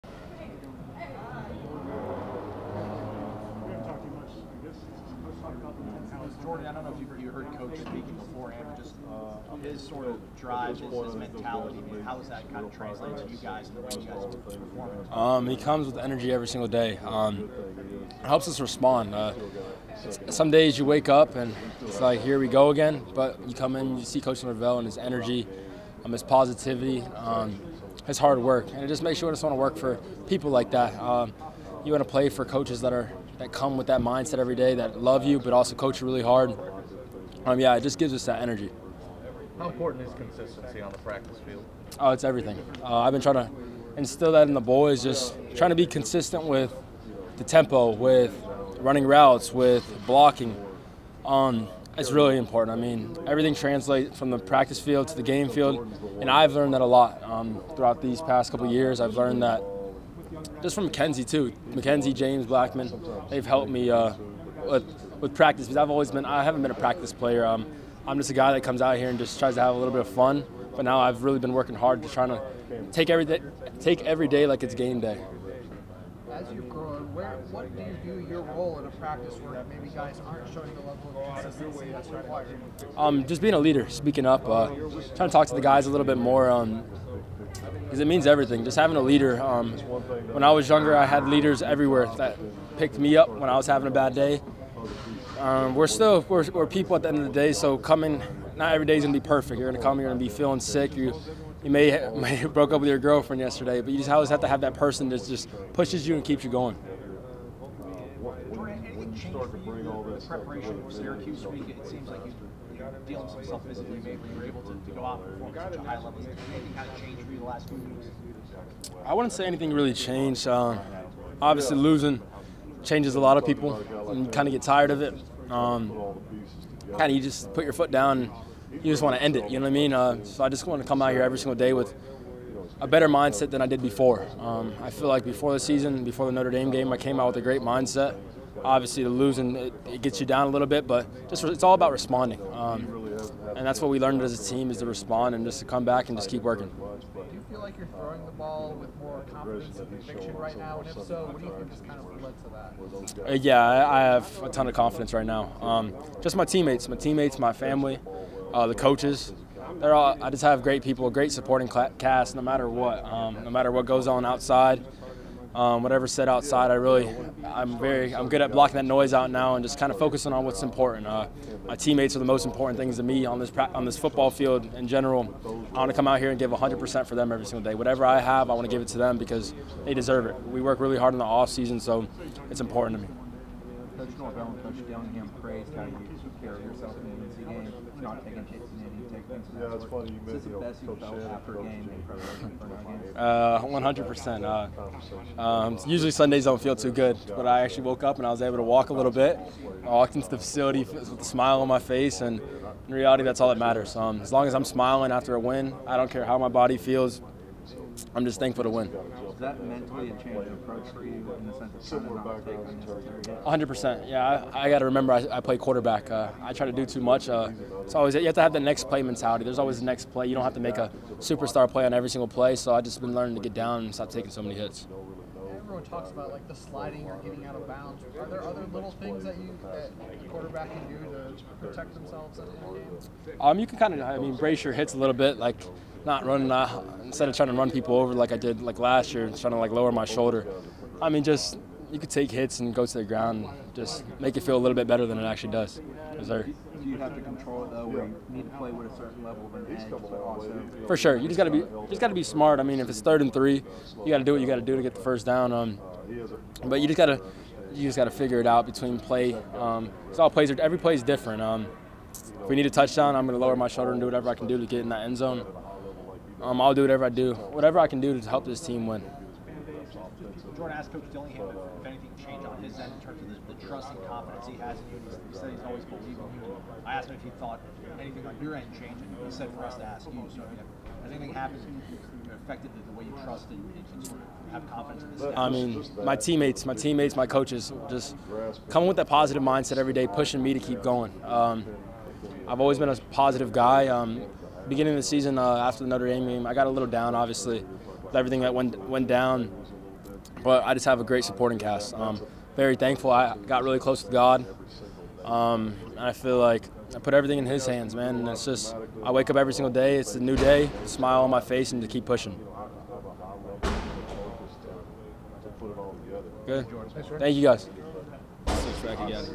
FSU QB Jordan Travis on budding confidence, health status
Travis opens about early season struggles as FSU prepares for UMass